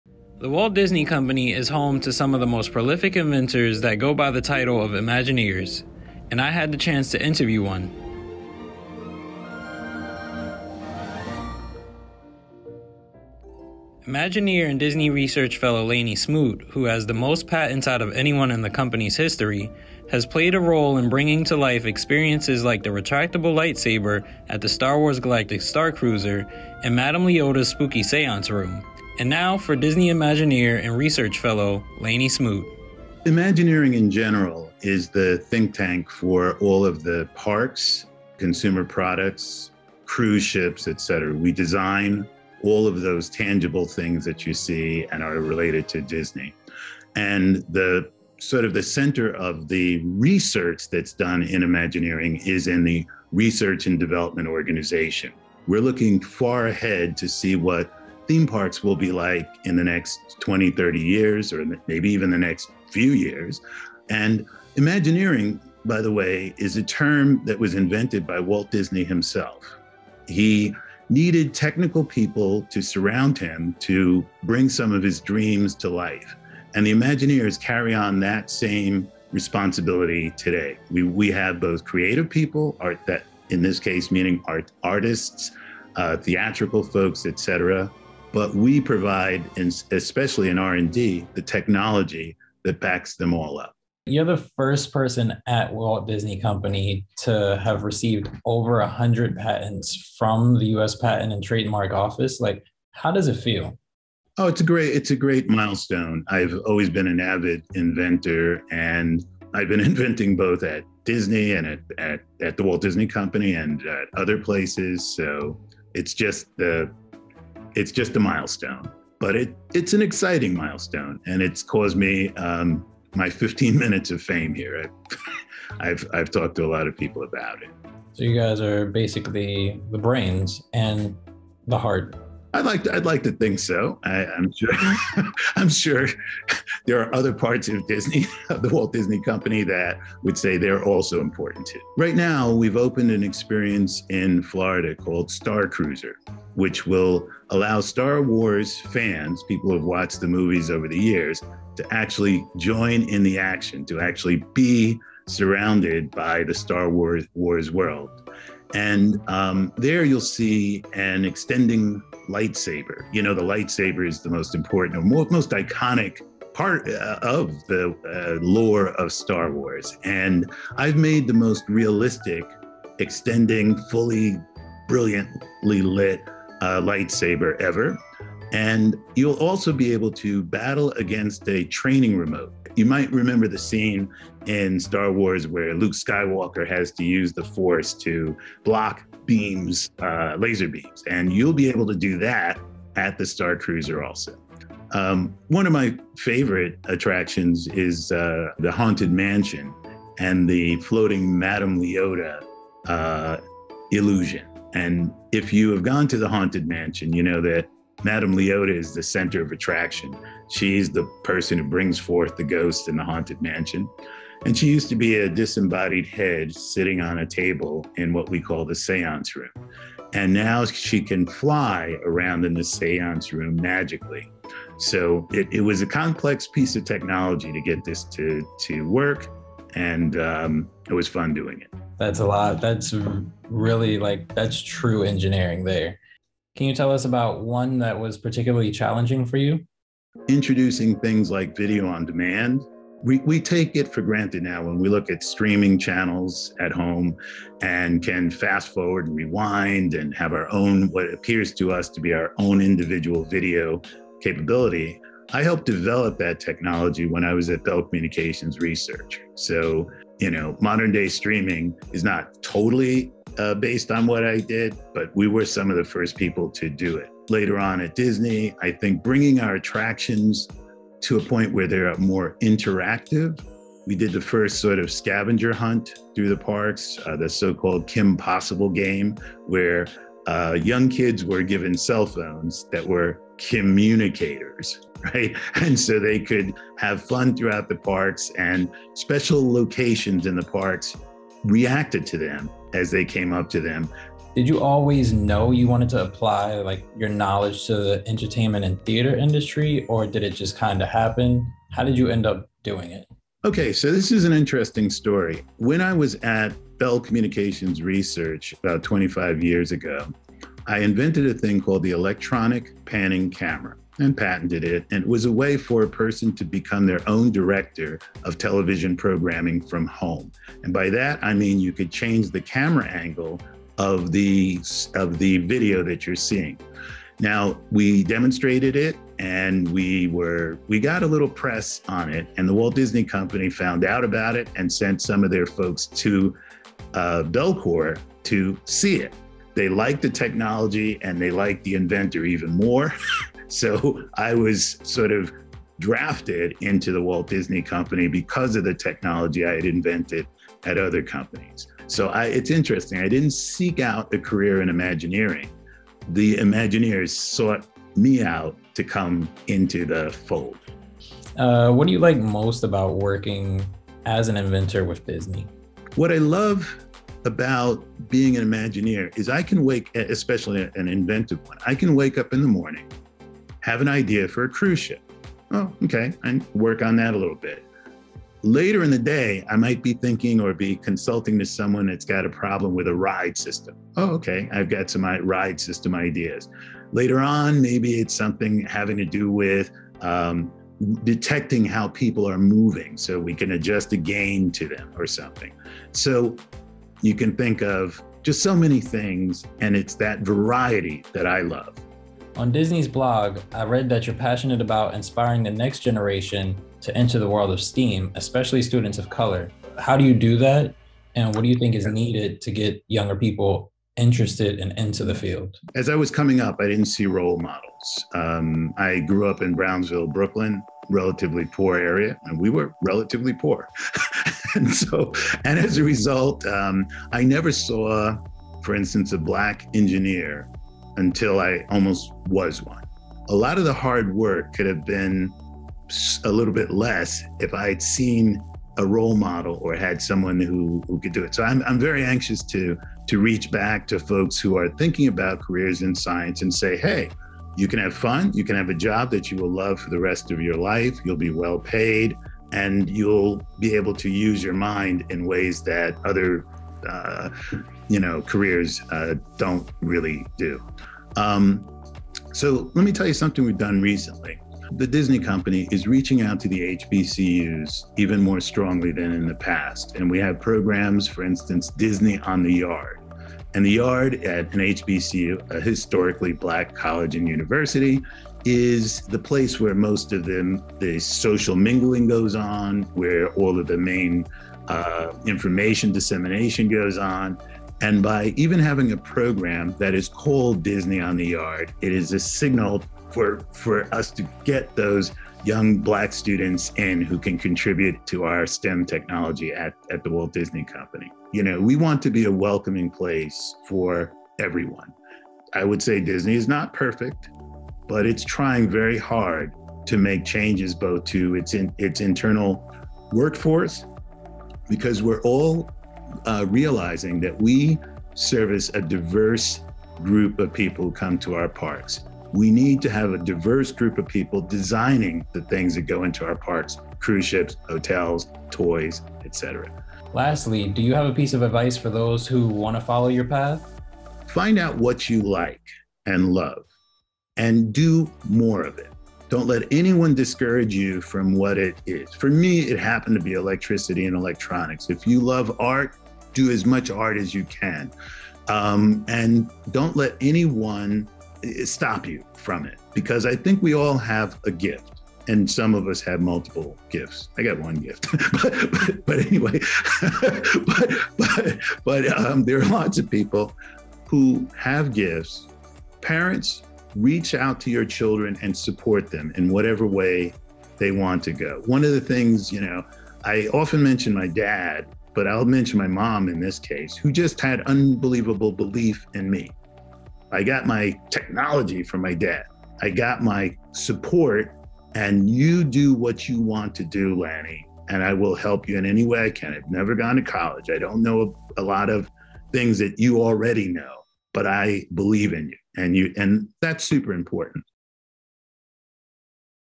Disney Imagineer Lanny Smoot Is the Man With Over 100 US Patents
The Walt Disney Company is home to some of the most prolific inventors in the US. They go by the title of imagineers. We sat down with Imagineer Lanny Smoot, who helped bring to life experiences like the retractable lightsaber on the Star Wars Galactic Starcruiser and Madame Leota's spooky séance room.